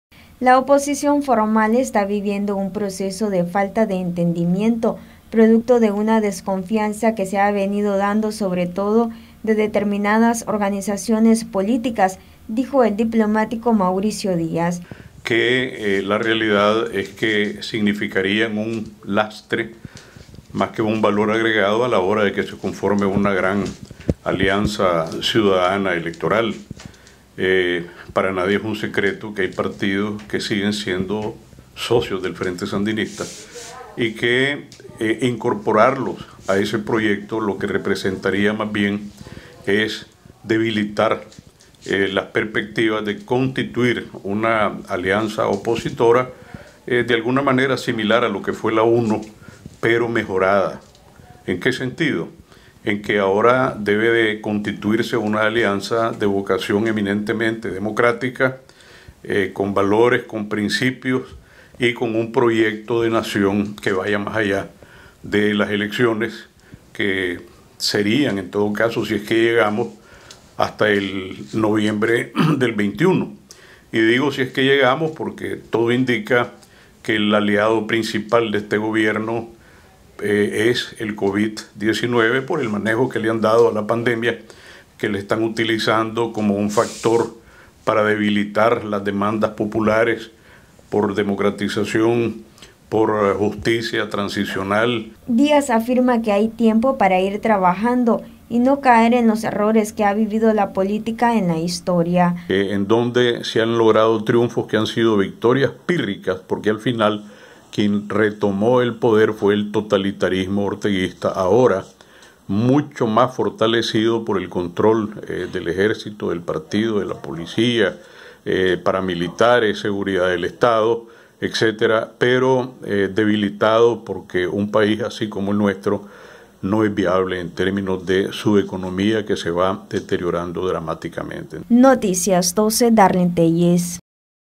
Díaz: Oposición está viviendo un proceso de falta de entendimiento
El diplomático Mauricio Díaz dijo que, aunque hay un proceso de falta de entendimiento por la oposición, hay tiempo para no caer en los errores que se han vivido en la historia.